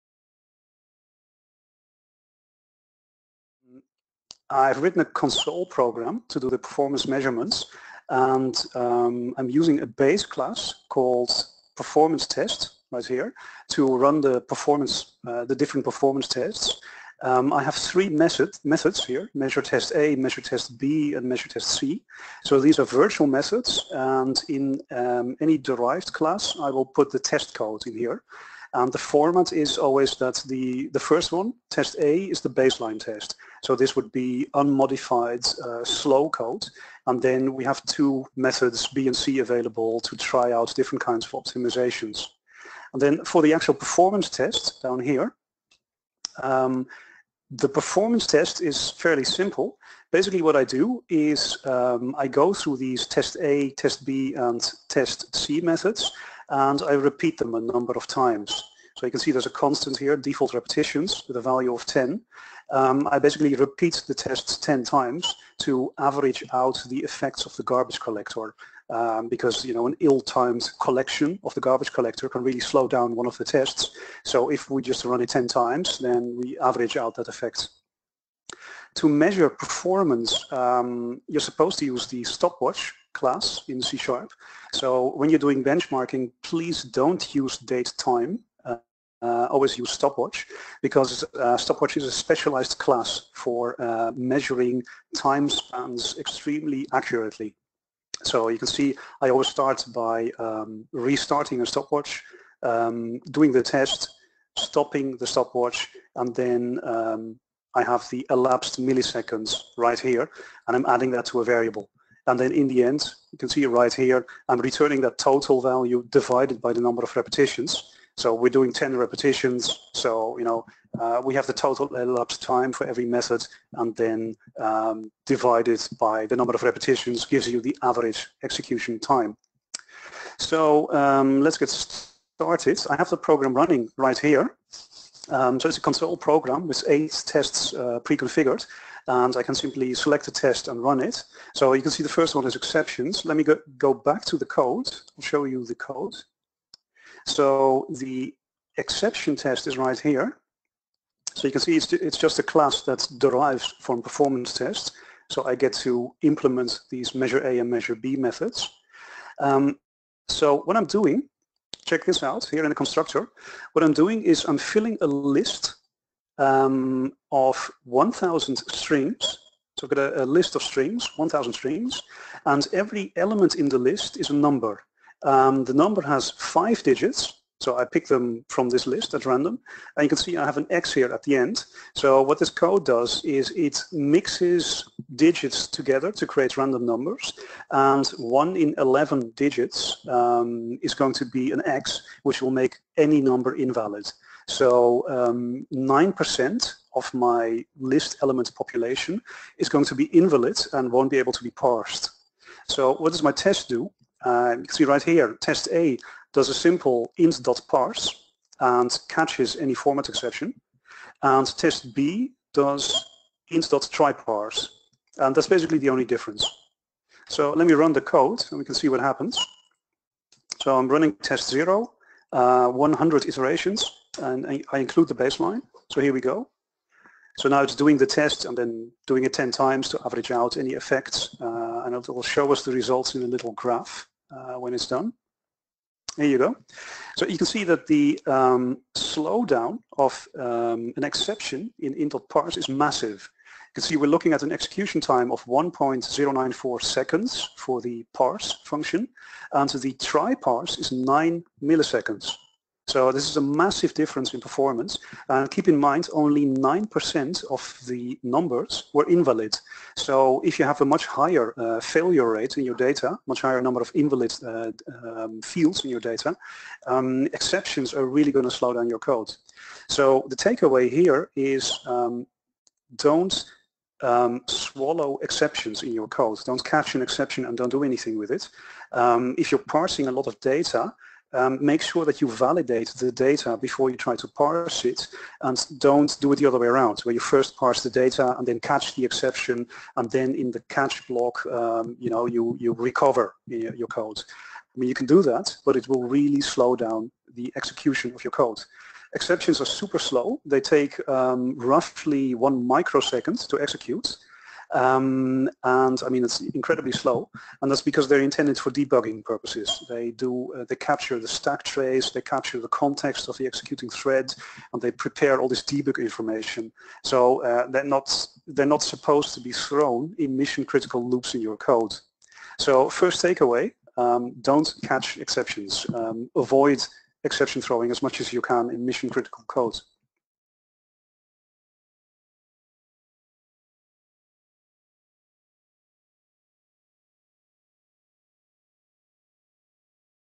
The exception penalty in C#
In this webinar, I demonstrate the performance penalty for throwing exceptions in C# code.